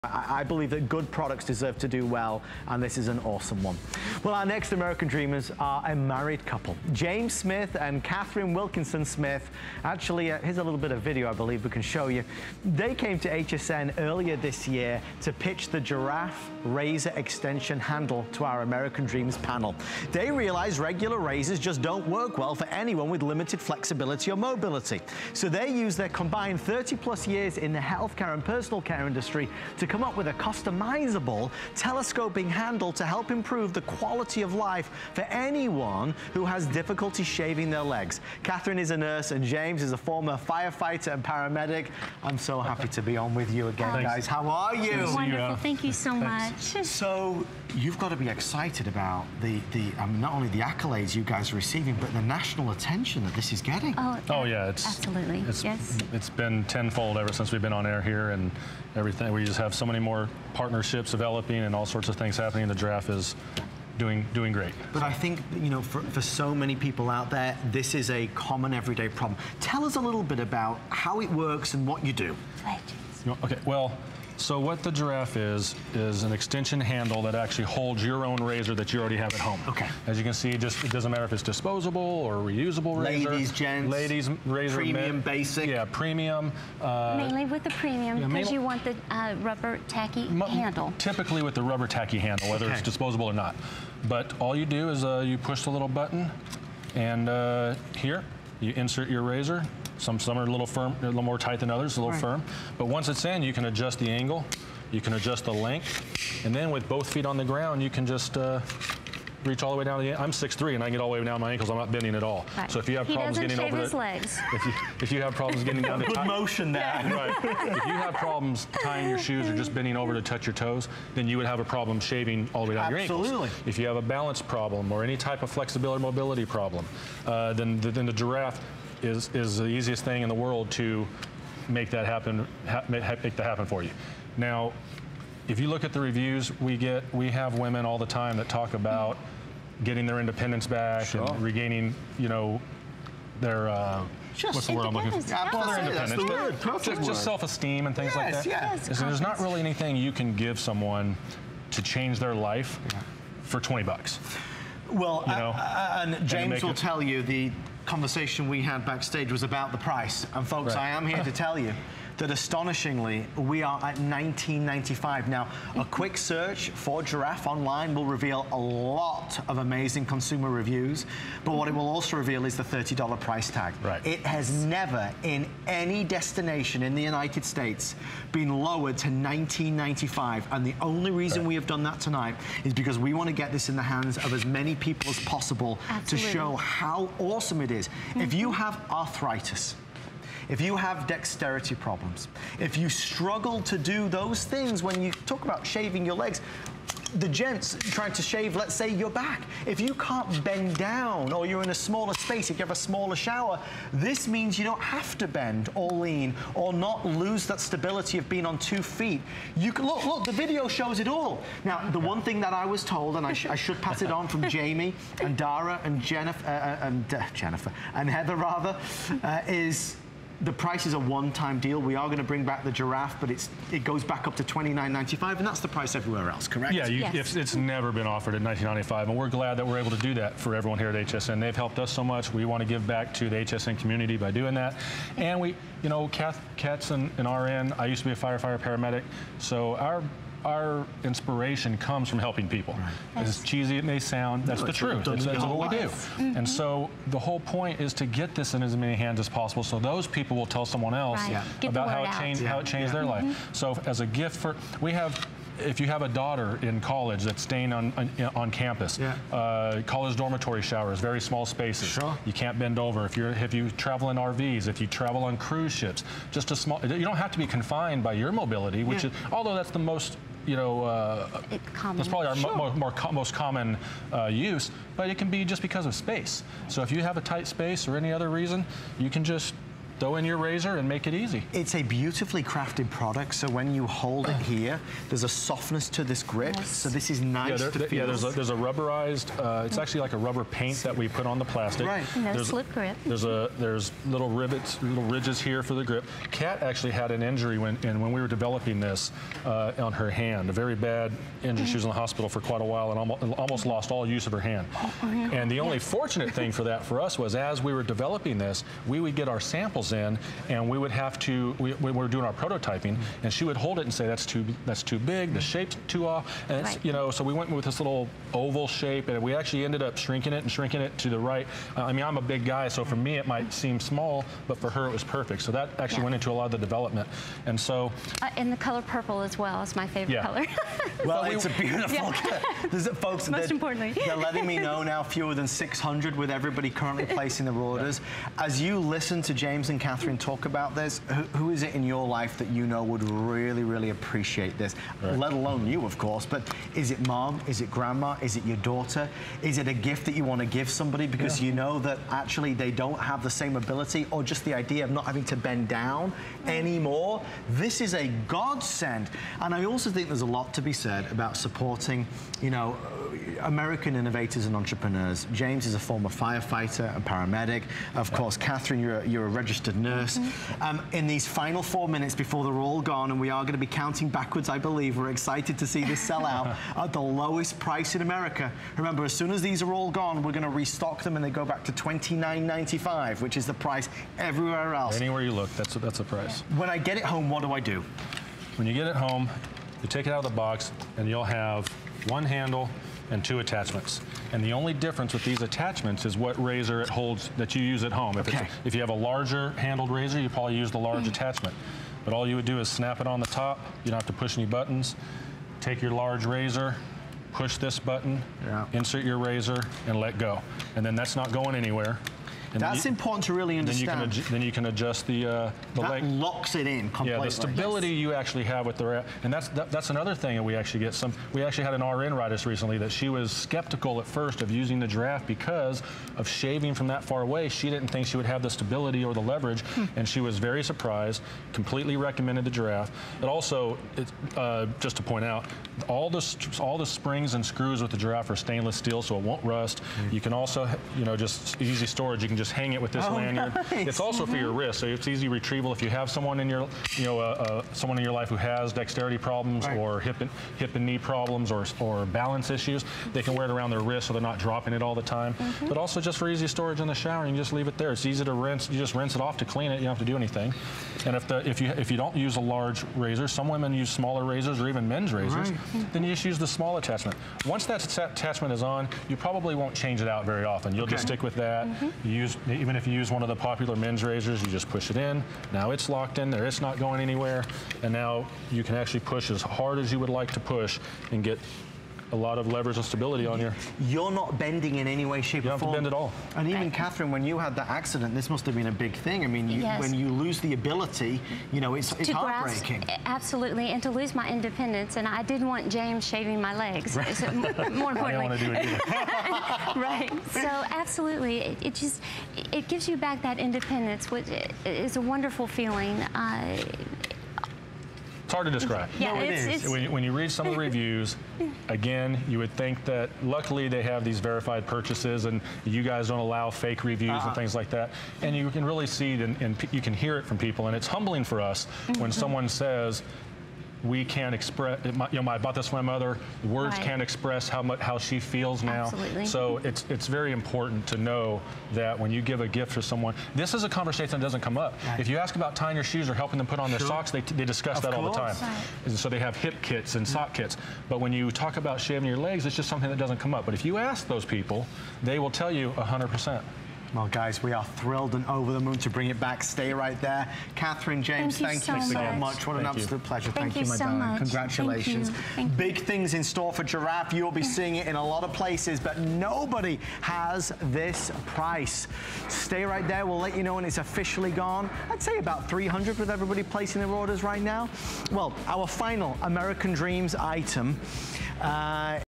The Giraffe Razor Extension Handle
Who knew your ankles could seem so far away when shaving? Instead of ditching your razor, give it some reach with this extension handle. Just lock it on your...